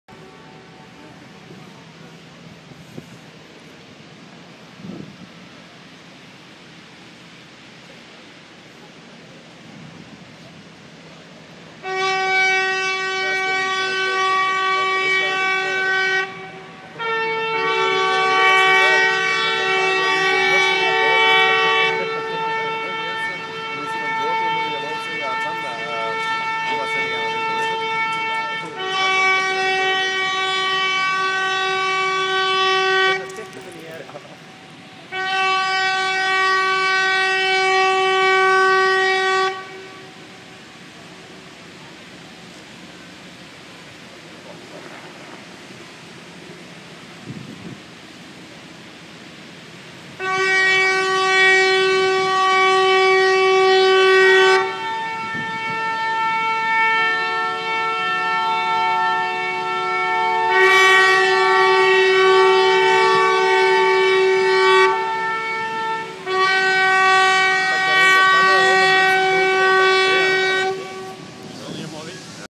URS tugs using fire fight
Union Panda, Koala using their FIFI instalation with Union Hawk in the background